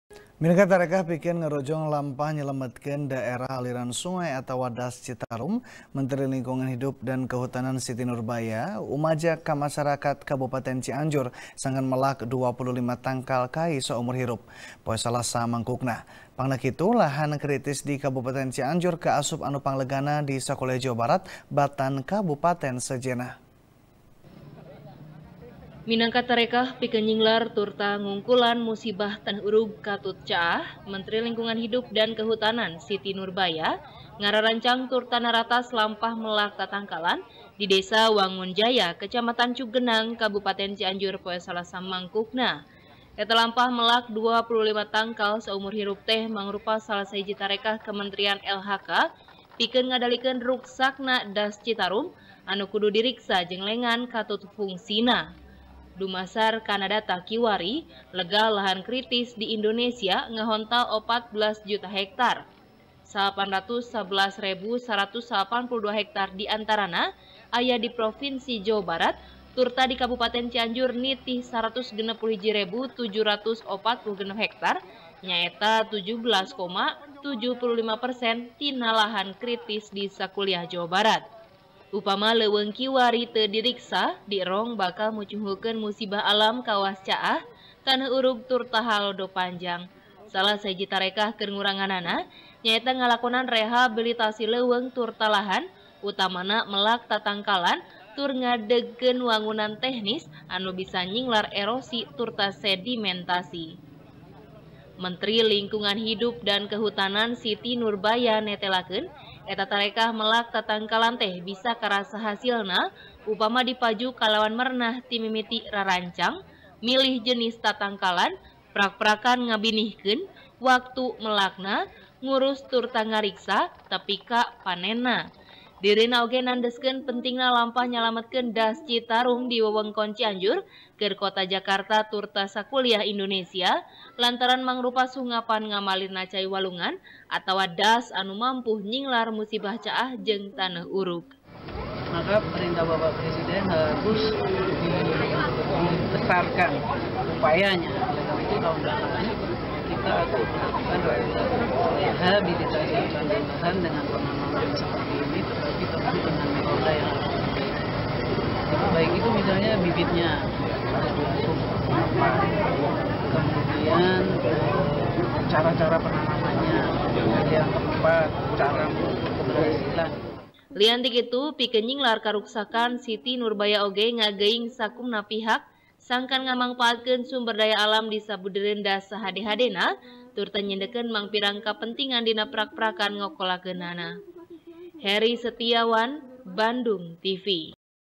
Menteri LHK Umajak Ka Warga Melak 25 Tangkal Saumur Hirup | TP | BANDUNGTV
TP | BANDUNGTV | DERI FIRMAN | Minangka tarekah pikeun ngarojong lampah nyalametkeun daerah aliran sungai atawa das citarum, menteri lingkungan hidup dan kehutanan siti nurbaya, umajak ka masarakat kabupaten cianjur sangkan melak dua puluh lima tangkal kai saumur hirup, poe salasa mangkukna. Pangna kitu, lahan kritis di kabupaten cianjur kaasup anu panglegana di sakuliah jawa barat, batan kabupaten sejenna.